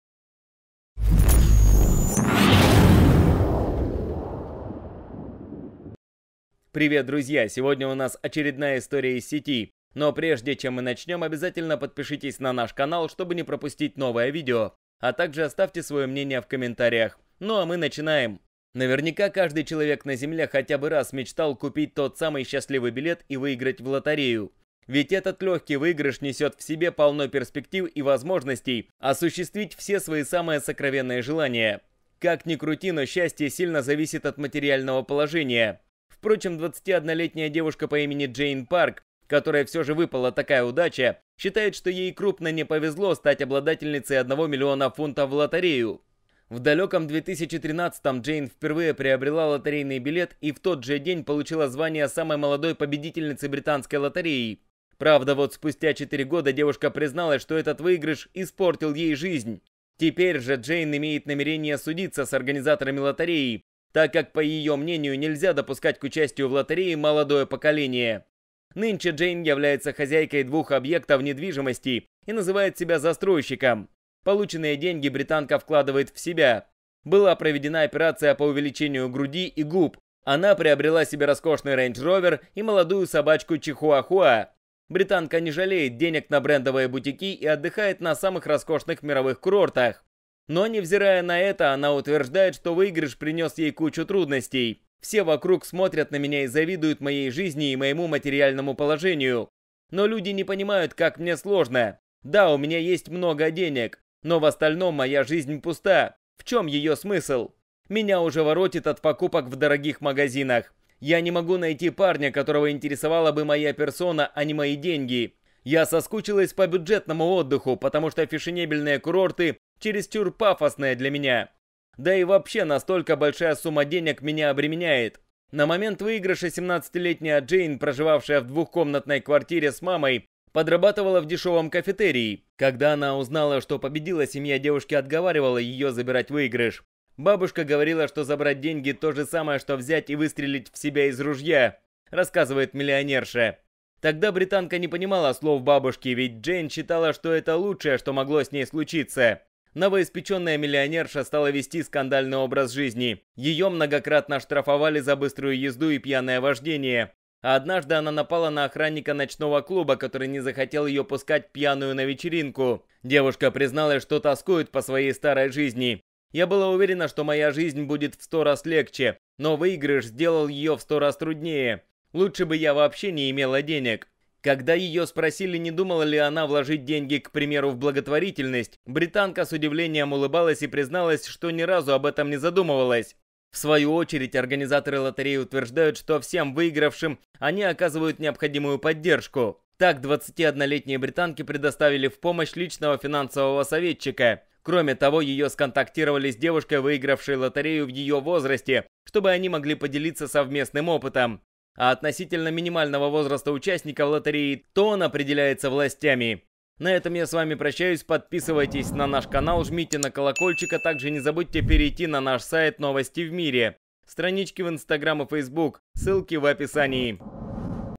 В 17 лет она выиграла миллион, а спустя 4 года подала в суд за то, что выигрыш сломал ей жизнь
Наверняка, каждый человек на Земле хотя бы раз мечтал купить тот самый счастливый билет и выиграть в лотерею. Ведь этот легкий выигрыш несет в себе полно перспектив и возможностей осуществить все свои самые сокровенные желания.